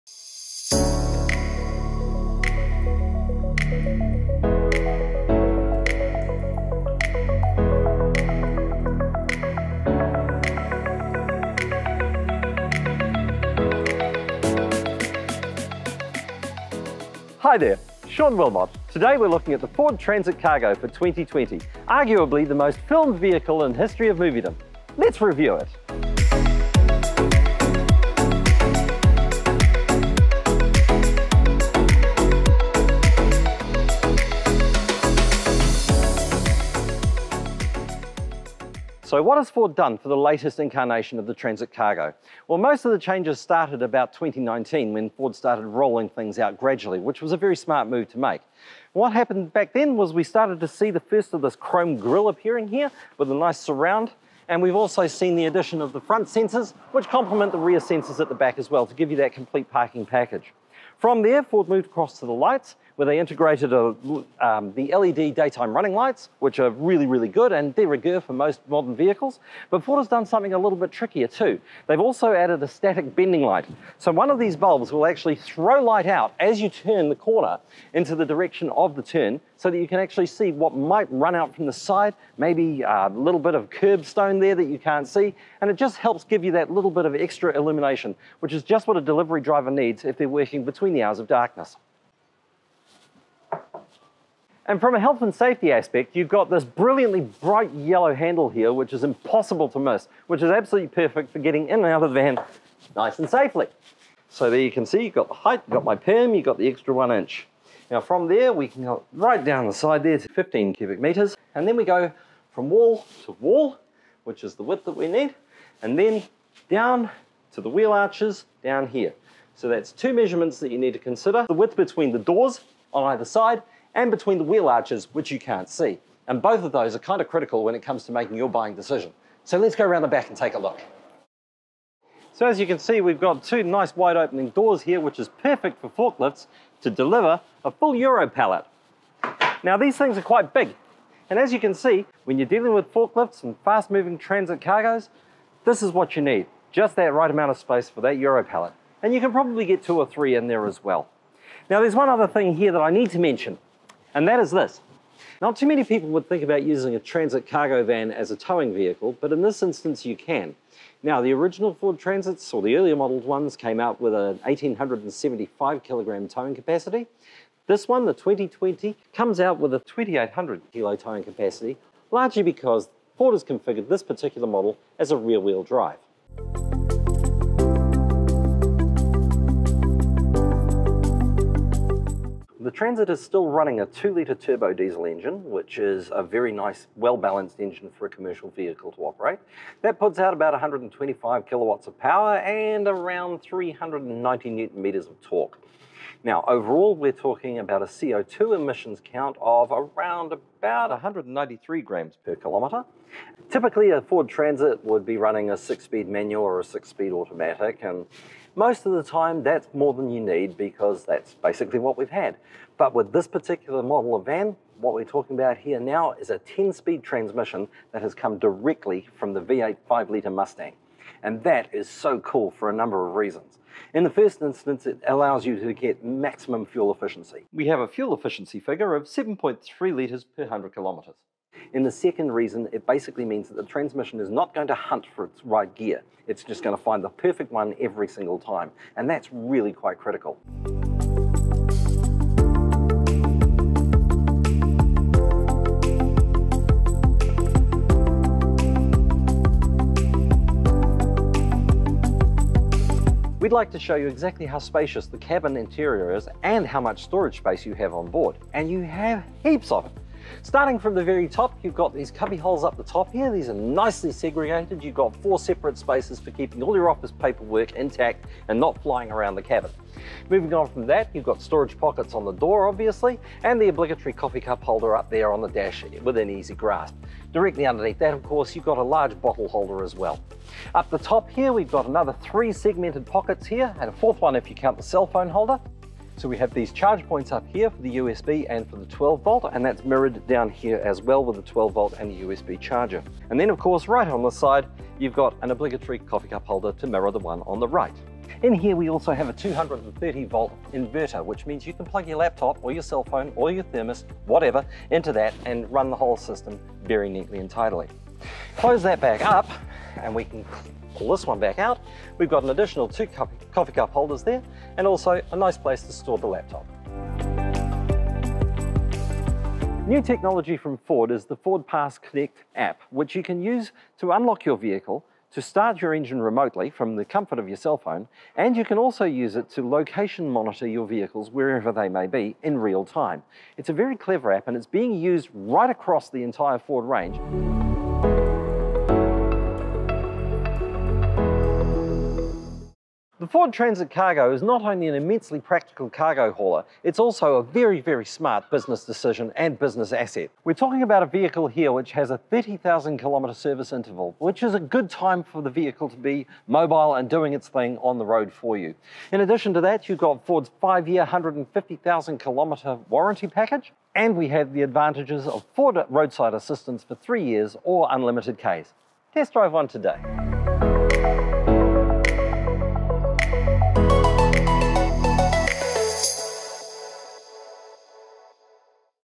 2021 Ford Transit Cargo
We video review the 2021 Ford Transit Cargo 350E High roof.

It’s 2021 and the dawn of a new drive for Ford’s Transit Cargo – quite literally. The versatile Ford Transit is now available with not only rear-wheel-drive, which increases towing capacity, provides better handling stability and ease of maintenance, it also comes with the impressive feature of a 10-speed automatic transmission. This is the same transmission you’ll find in the 5-litre Mustang, which for the Transit, means better fuel economy and ease of driving with less stress on the already capable 2-litre turbo diesel.